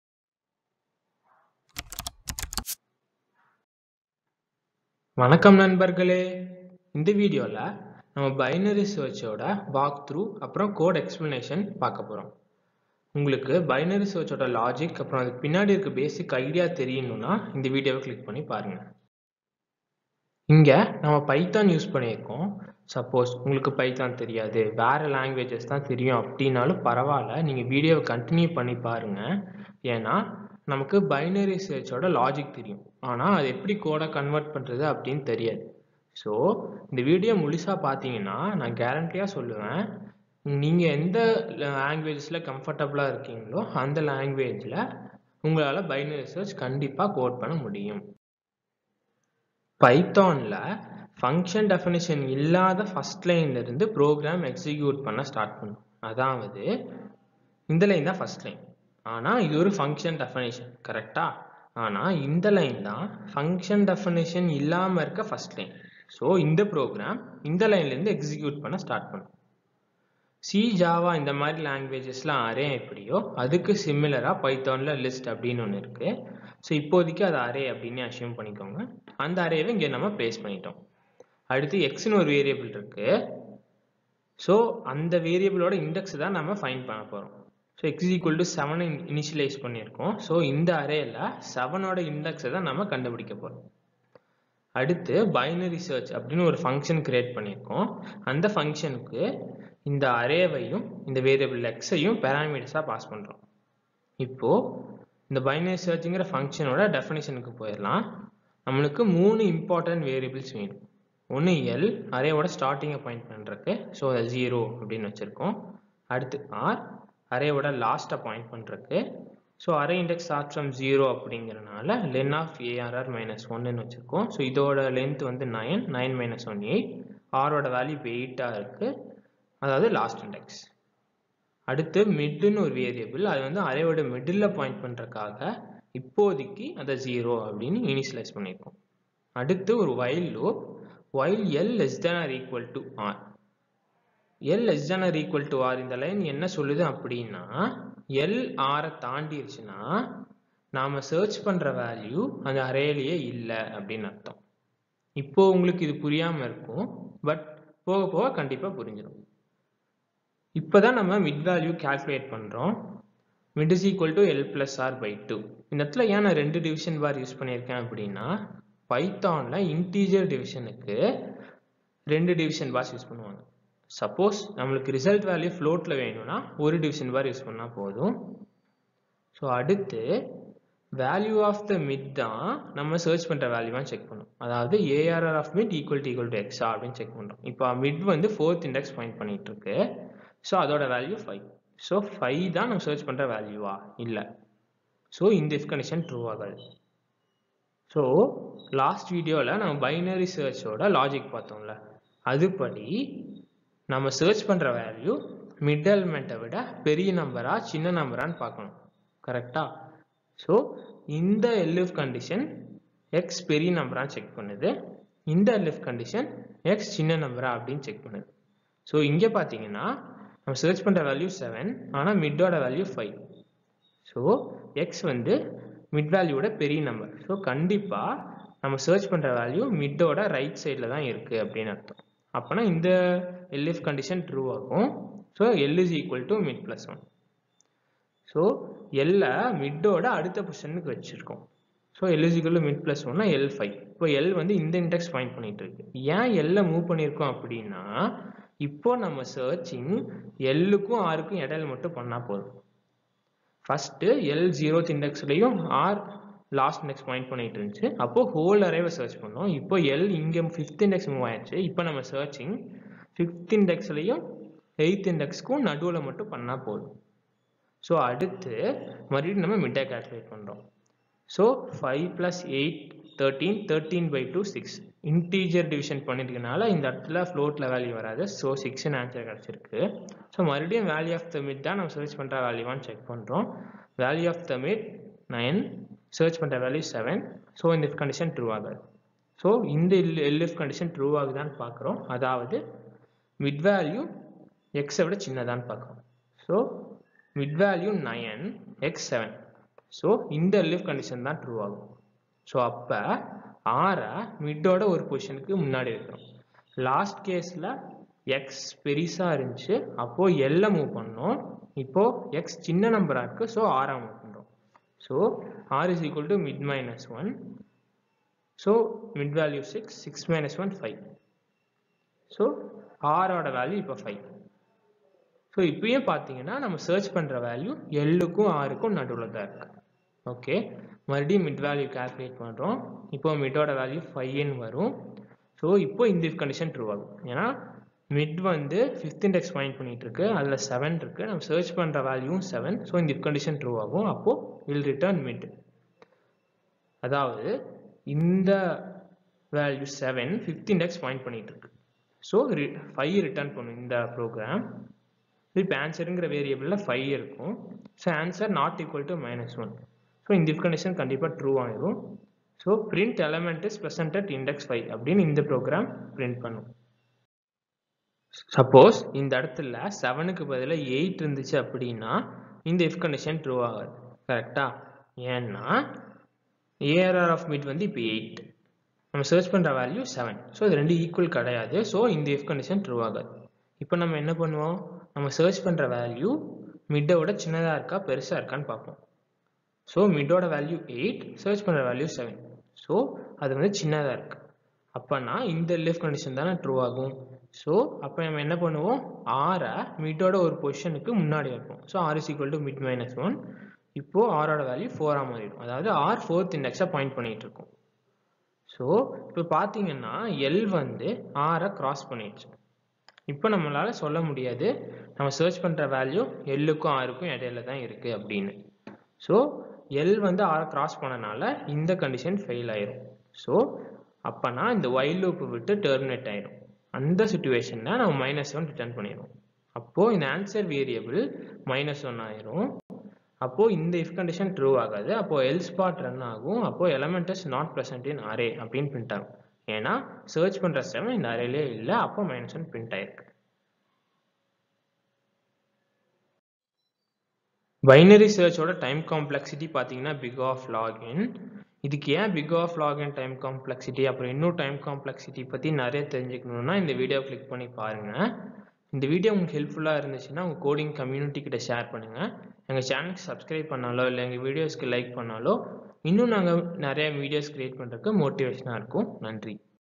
BINARY SEARCH | PART 2 | TAMIL | CODE EXPLANATION | WALKTHROUGH | ANIMATION
In this video we clearly explained binary search with little animation.
This whole video is made using powerpoint  🤗.